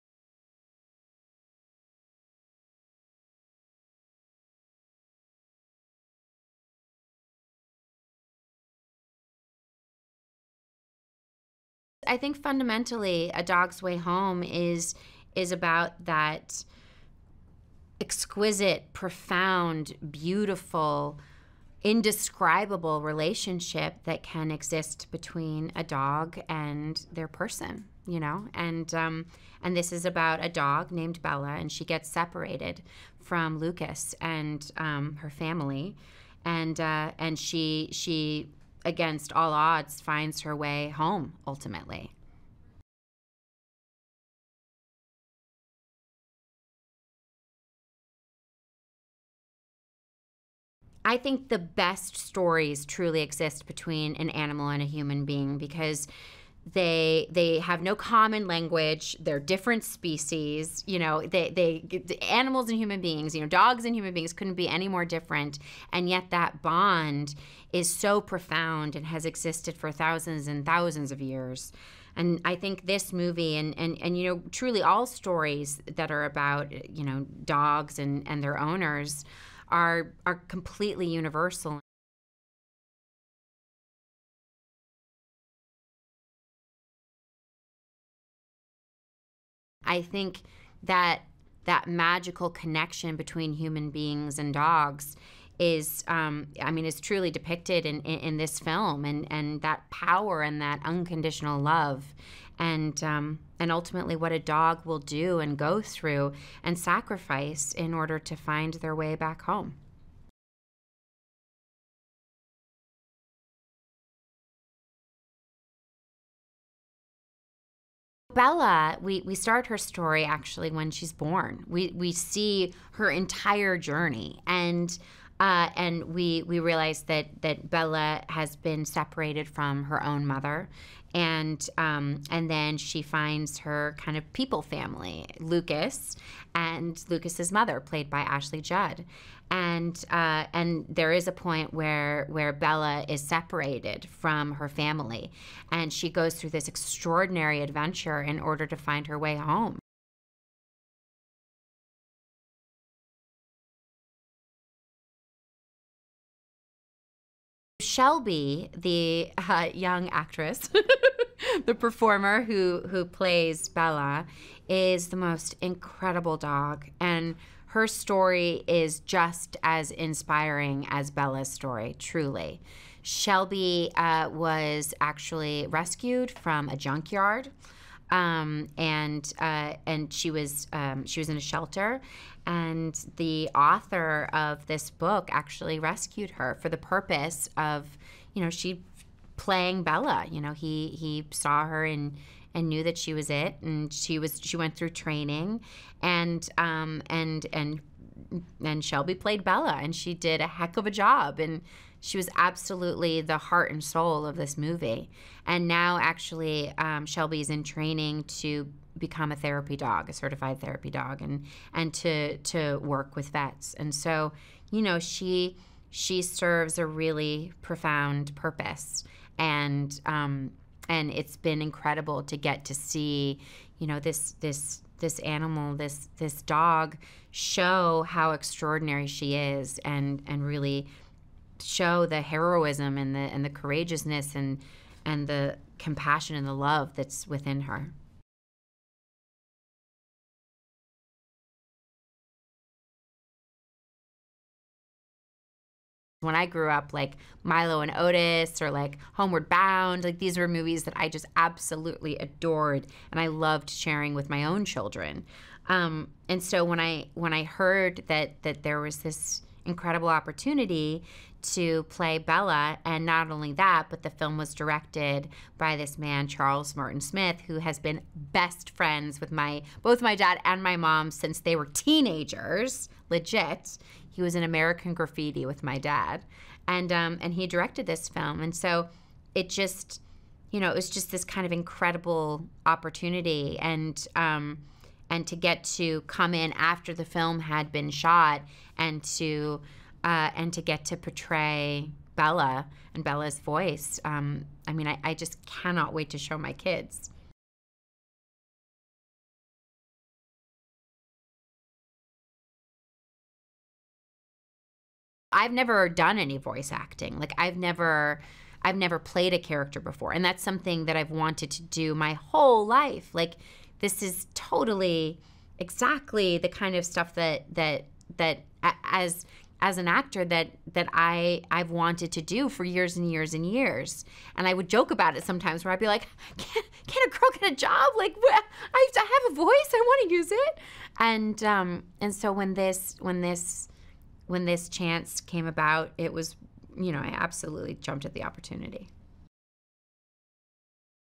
A Dog's Way Home || Bryce Dallas Howard - "Voice of Bella" Junket Soundbites
Watch A Dog's Way Home  Bryce Dallas Howard - "Voice of Bella" Junket Soundbites

As a puppy, Bella finds her way into the arms of Lucas, a young man who gives her a good home. When Bella becomes separated from Lucas, she soon finds herself on an epic, 400-mile journey to reunite with her beloved owner.
Starring: Ashley Judd,Bryce Dallas Howard, Jonah Hauer-King, Alexandra Shipp, Wes Studi, Edward James Olmos
Director: Charles Martin Smith
Based on: A Dog's Way Home; by W. Bruce Cameron
Screenplay: W. Bruce Cameron, Cathryn Michon
Production companies: Columbia Pictures, Polybona Films, Pariah
Producers: W. Bruce Cameron, Gavin Polone